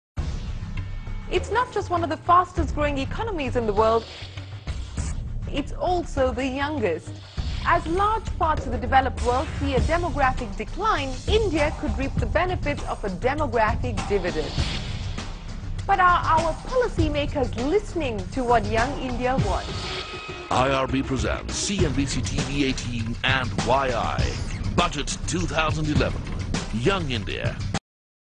SSE.mpg
India's working population is brimming with youth and their boundless energy. Their passion and vision is the driving force behind India's growing ecomony. Young India a forum where India's young leaders and entrepreneurs voice their opinions.